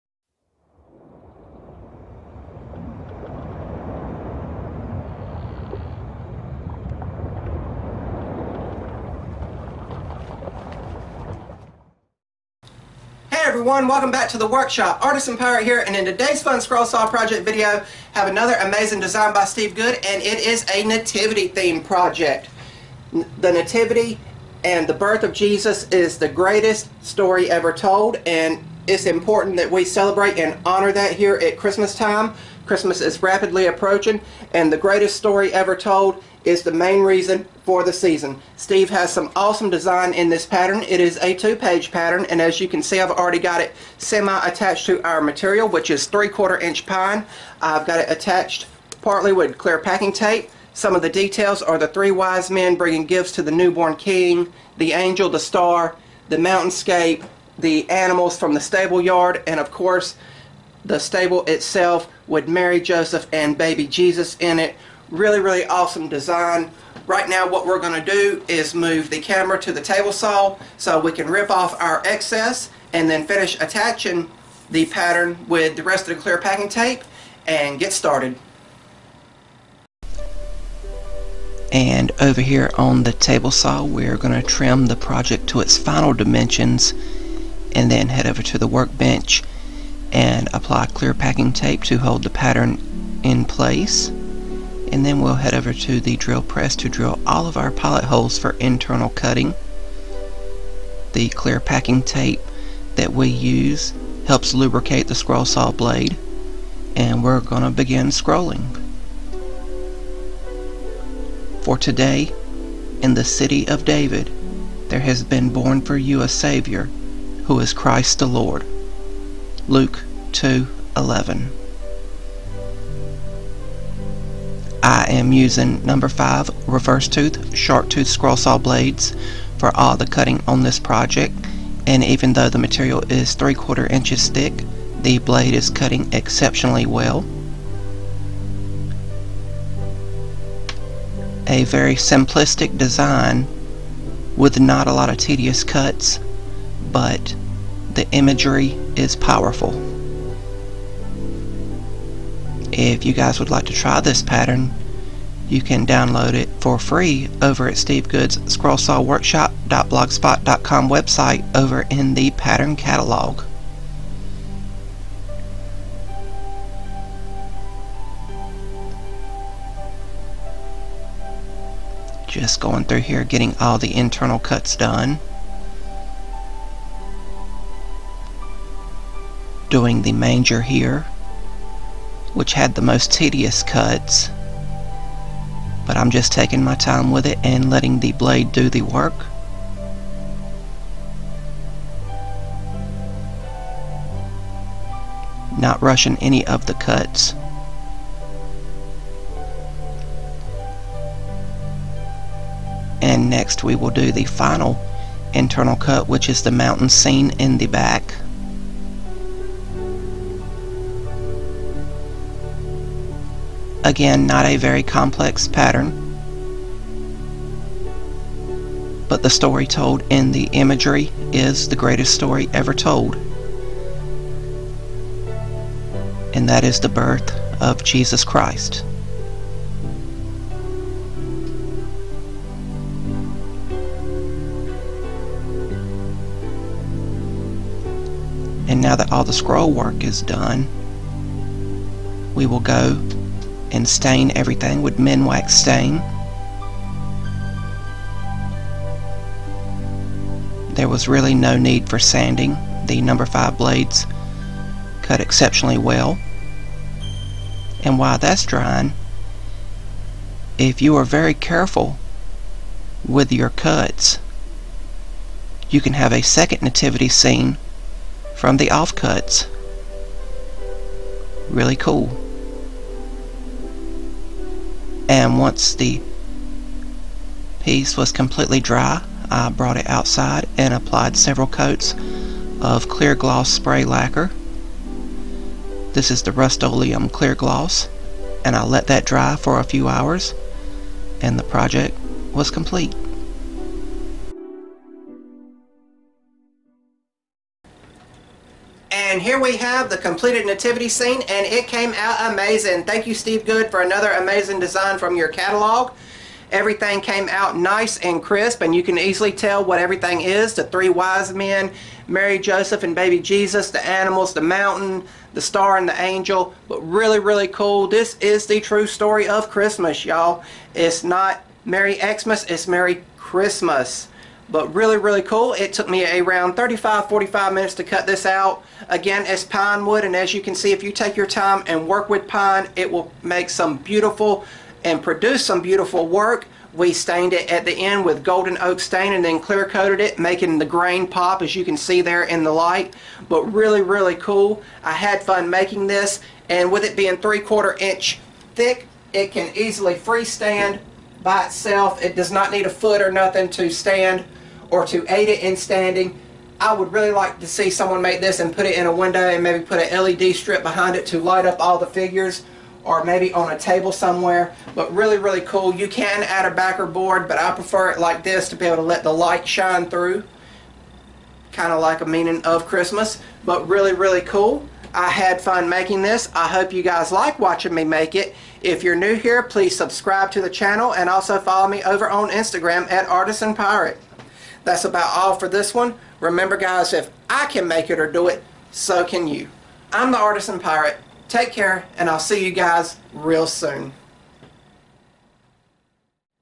Nativity Scene, Scroll Saw Project
Hey everyone!  In this Scroll Saw Project Video I make a beautiful Nativity Scene out of 3/4 inch pine.  This Steve Good design celebrates the actual true meaning of Christmas.  Hope you all like the video !

Remember, If I Can Make It Or Do It So Can You !
God Bless !

Mailing Address;